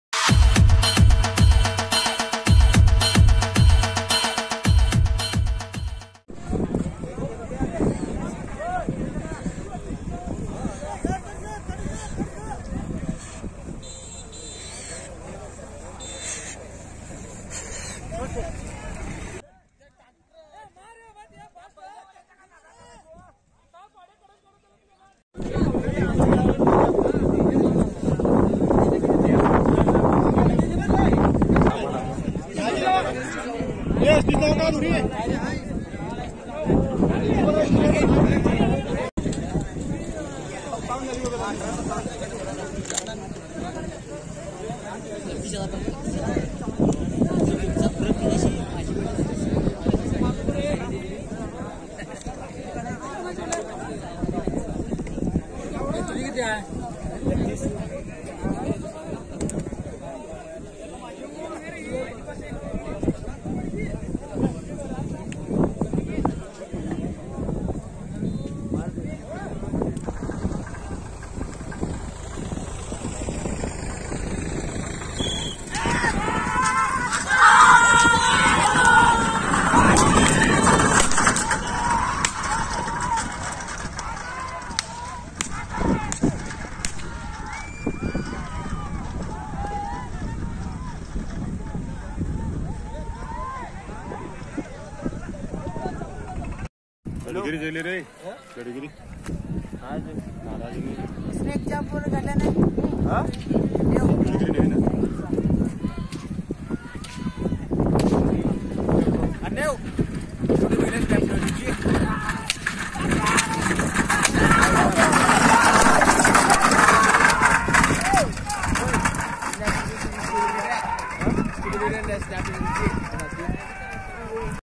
|| Jabardast Racing Bulls Running in Bailgada Sharyat || Ox Race ||
In this video you can watch racing bulls of Kudnur running in bullock cart race at Kinaye Belgaum.